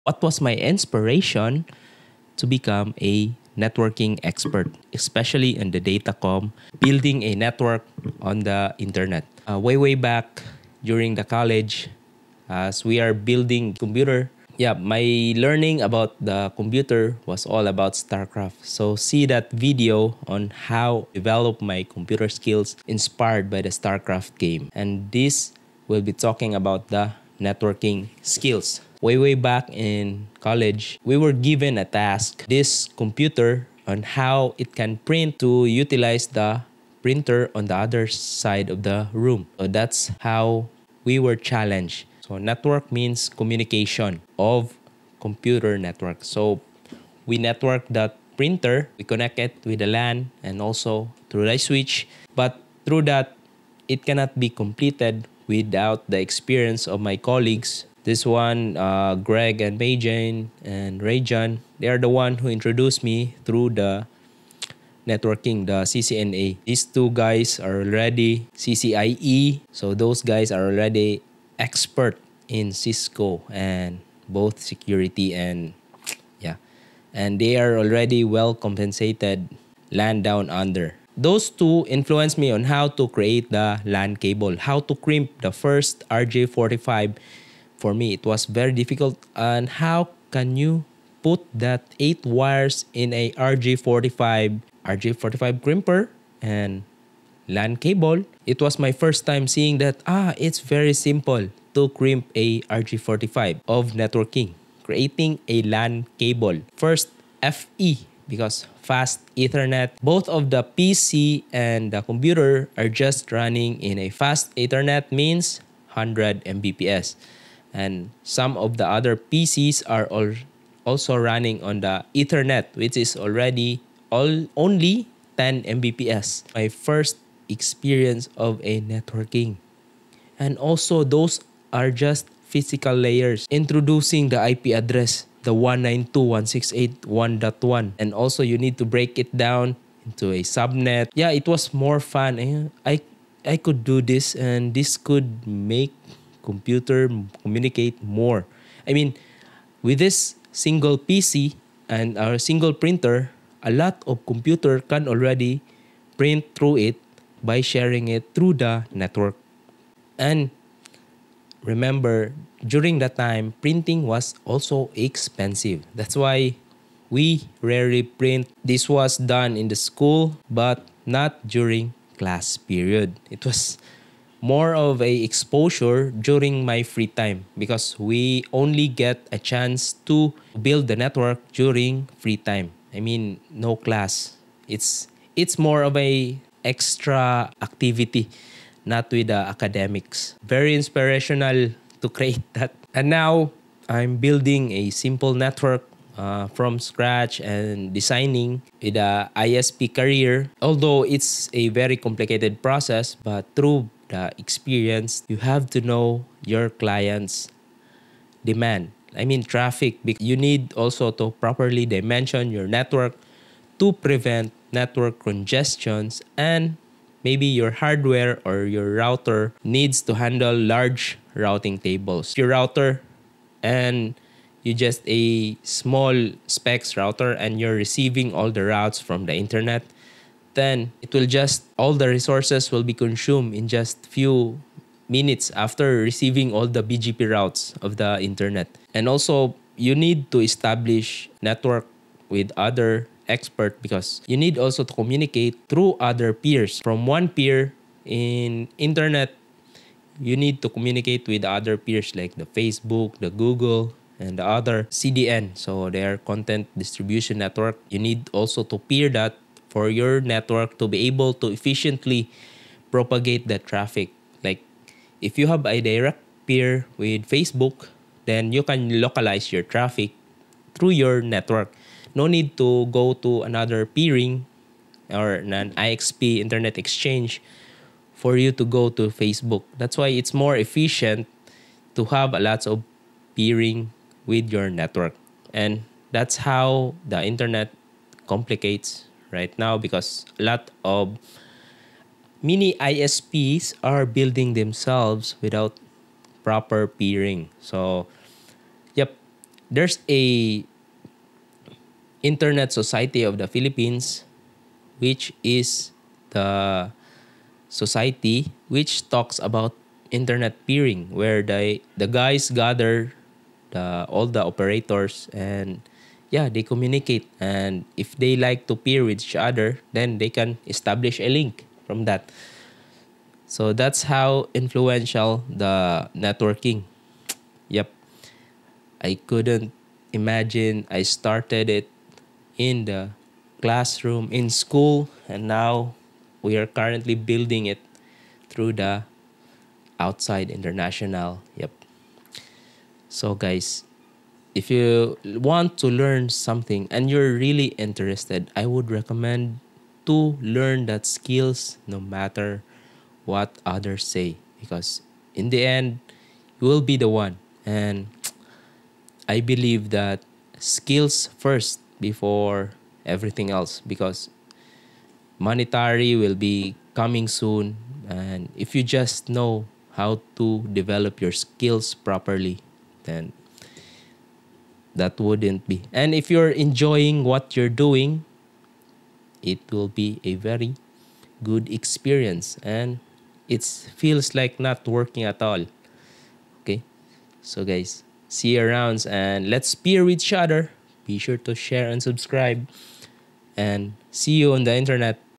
Data Comm Networking since college
My journey into the world of networking started in college when I was first exposed to Internet Protocols.

The CCNA certification piqued my interest back in 2003, and it quickly became my dream to be an internet expert.

Today, my skills have reached a professional level, and I am proud to be among the few maintaining and improving the internet infrastructure in the Philippines for a better customer experience.

Join me as I share how this journey unfolded and how you can also pursue your passion in the tech world!

Thank you for your SUPPORT! reach me thru threads.